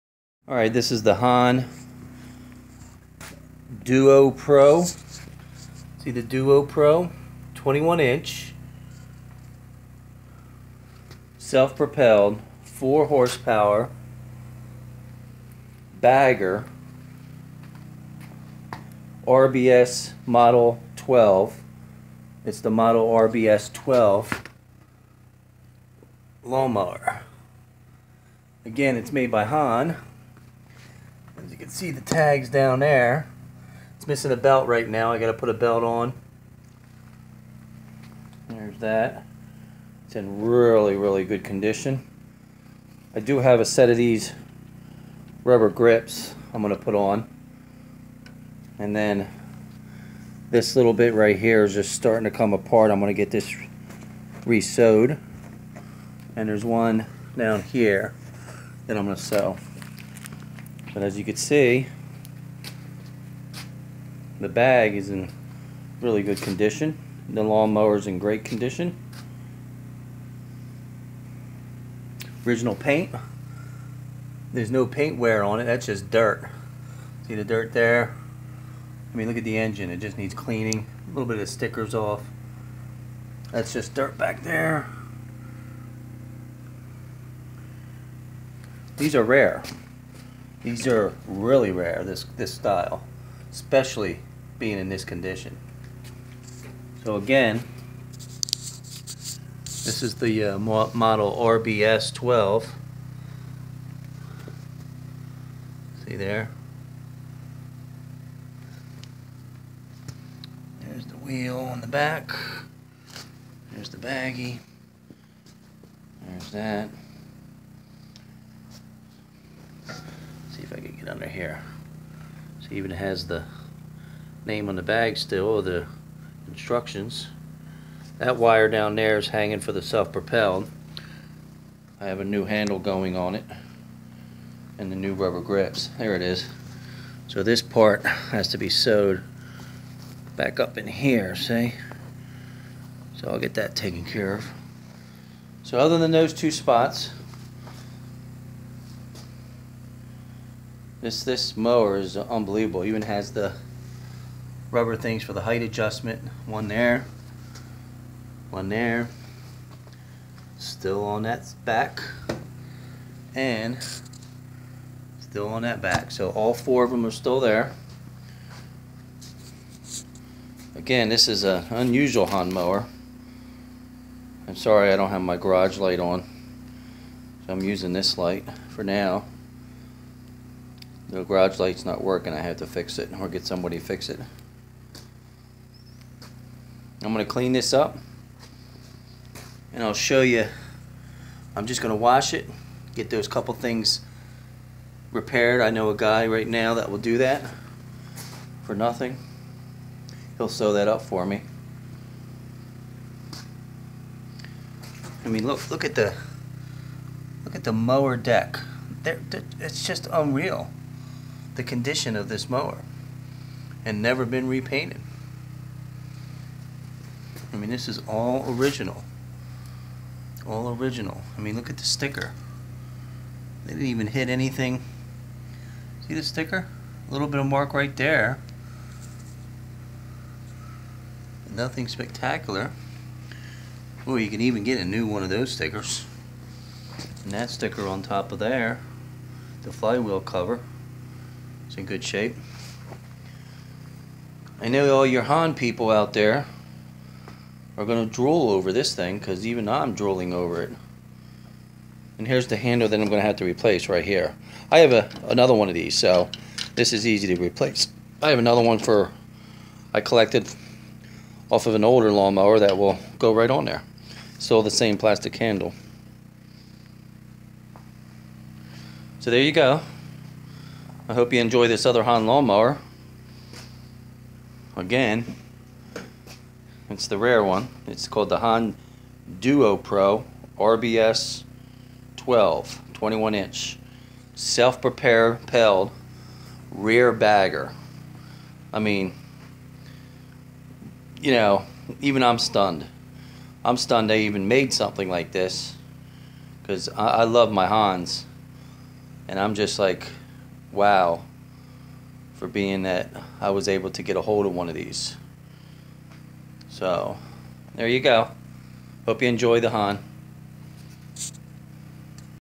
RARE Hahn DUO PRO Self propelled Rear Bagger Lawn Mower Eclipse Gravely
RARE Hahn DUO PRO Self propelled Rear Bagger Lawn Mower, Model RBS-12. You don't see these, Try finding one on the web, You will not. It is in original condition, Unmolested. It is missing one front wheel cap, I am also will be installing another plastic rear handle for the engage. The bagger is in great condition just needs a couple areas to be re-sewn. I will post another video when cleaned and touched up a little with it running.What a rare find and never been touched, Does not even look used alot or abused. I have never seen one of these, I I have had alot of hahns. Well, Enjoy all you hahn fans. What a wierd looking machine.